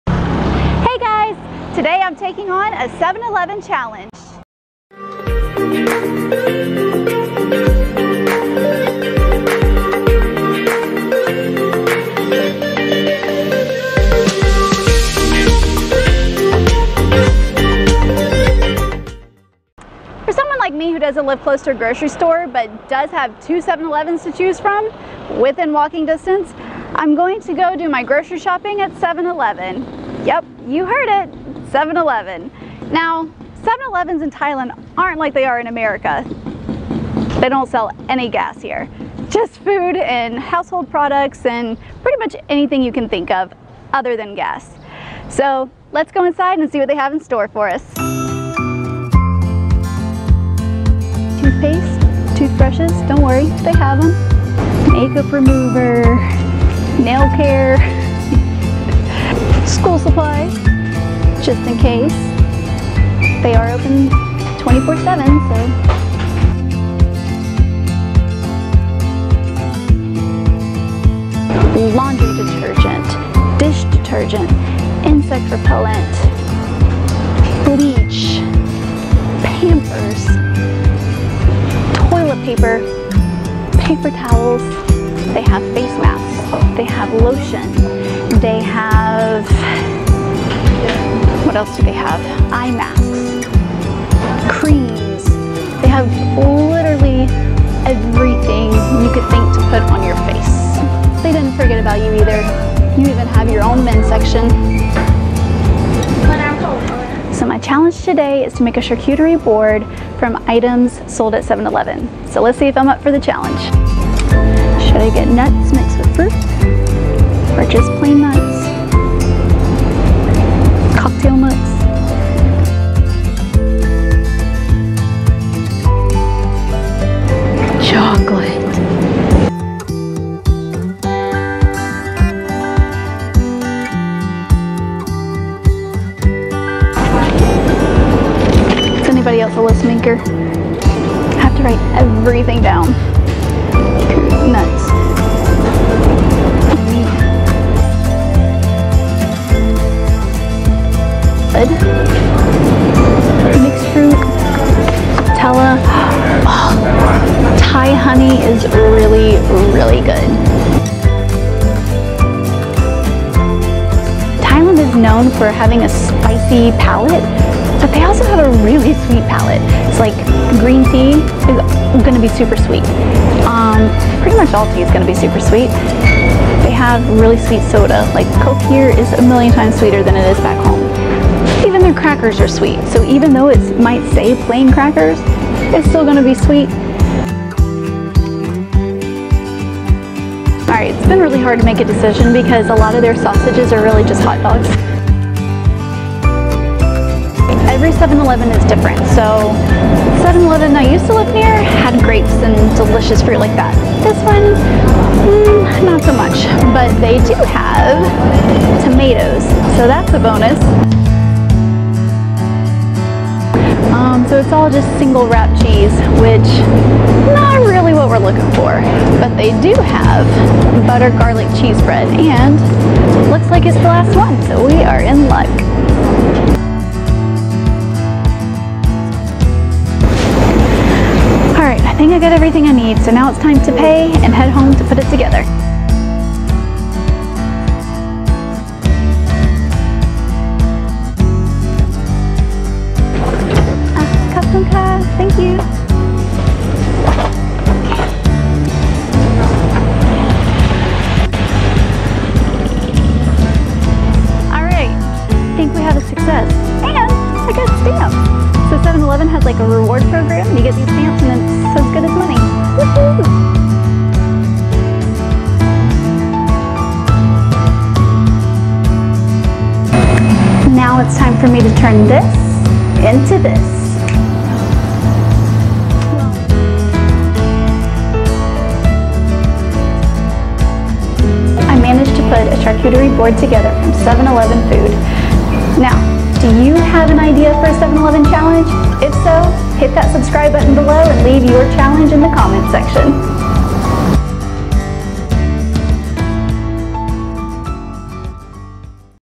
7- Eleven Challenge: Charcuterie Board • I’m Jess A Girl
7 Eleven is a necessity for every neighborhood in Thailand. But, I betcha didn't know how versatile they could be! Watch my journey to create a charcuterie board from items sold at 7 Eleven.